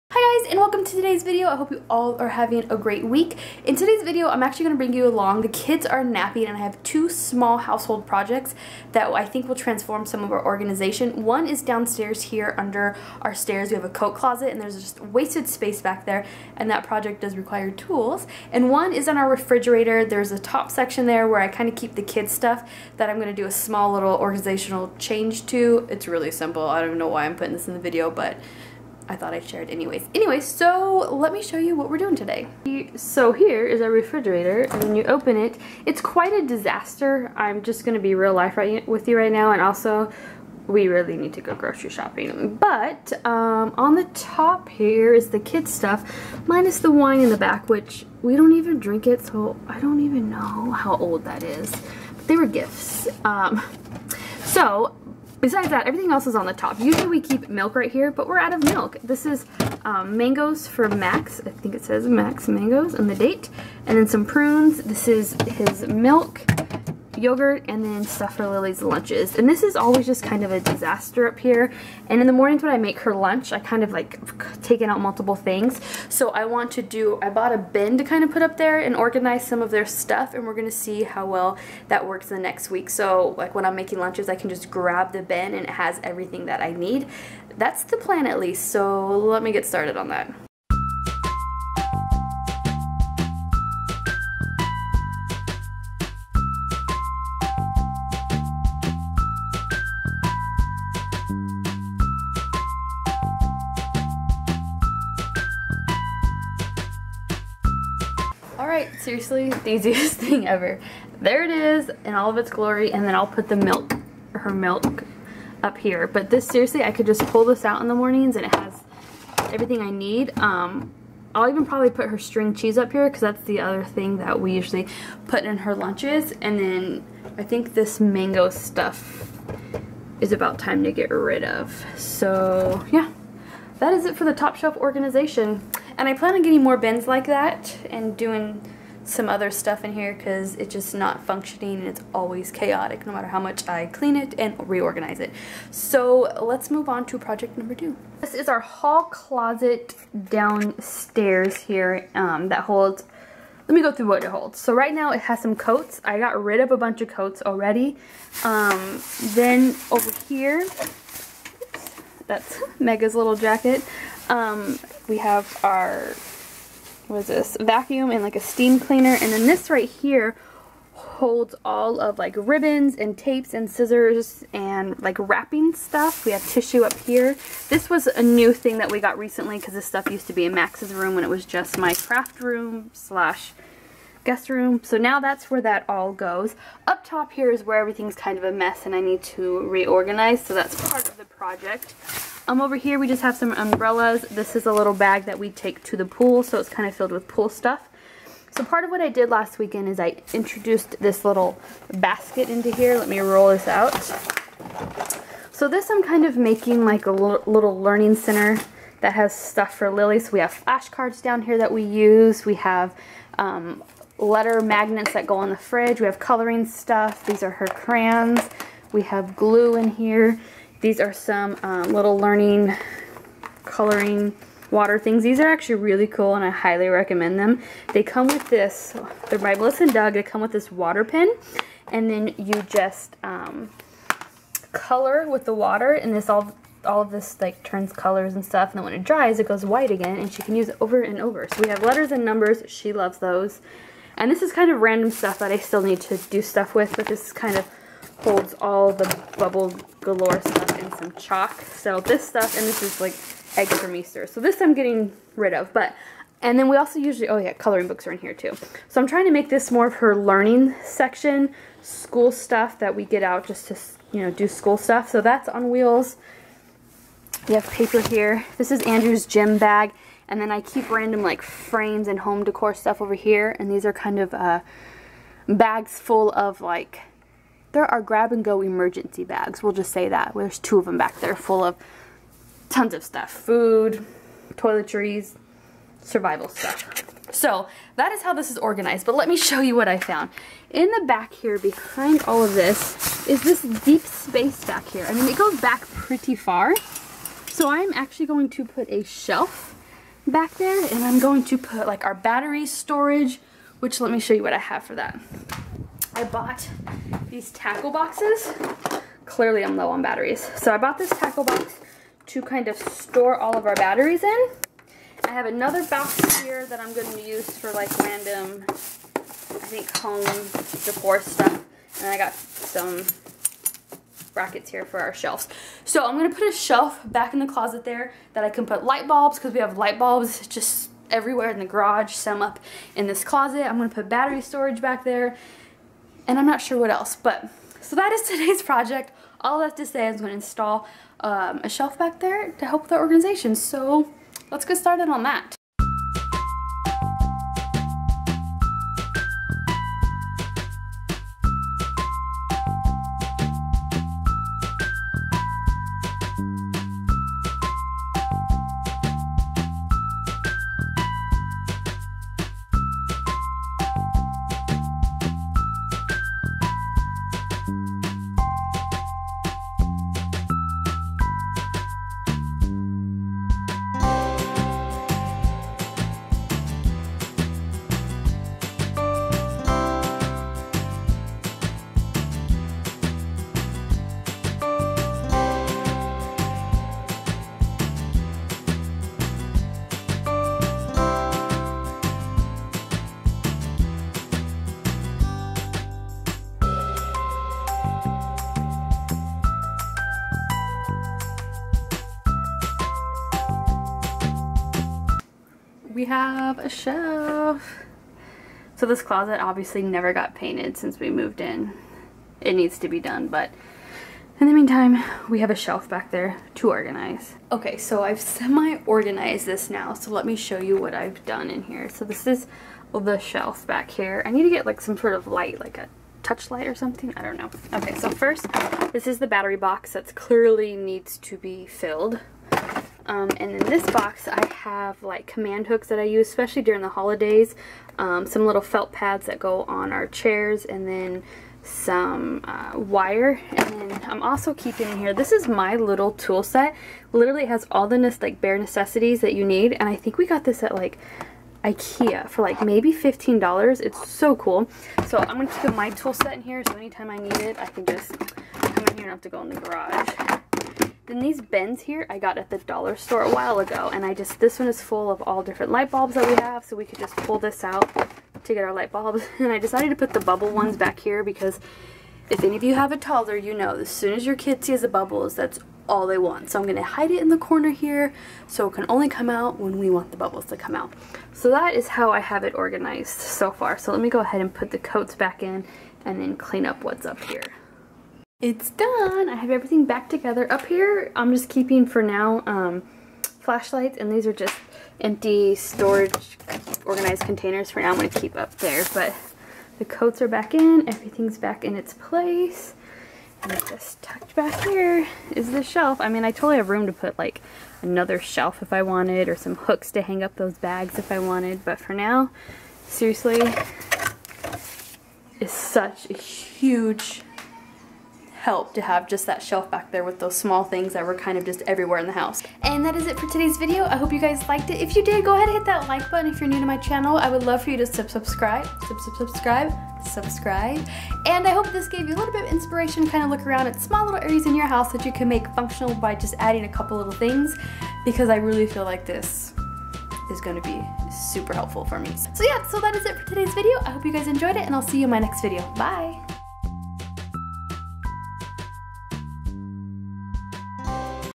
Home Organization Project | DIY Home Projects
I feel like life is a little disorganized so I am on a mission one project at a time to get our house organized. I decided to bring you along on 2 of those projects!
Jennifer